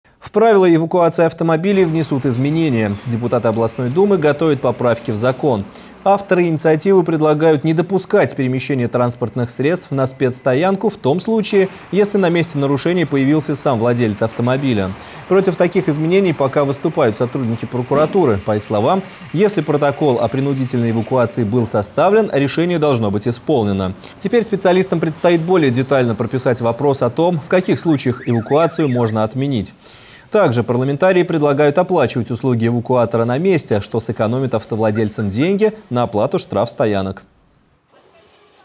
В правила эвакуации автомобилей внесут изменения
Депутаты Областной Думы готовят поправки в закон. Авторы инициативы предлагают не допускать перемещение транспортных средств на спецстоянку в том случае, если на месте нарушения появится сам владелец автомобиля. Подробнее смотрите в нашем материале от 2 сентября.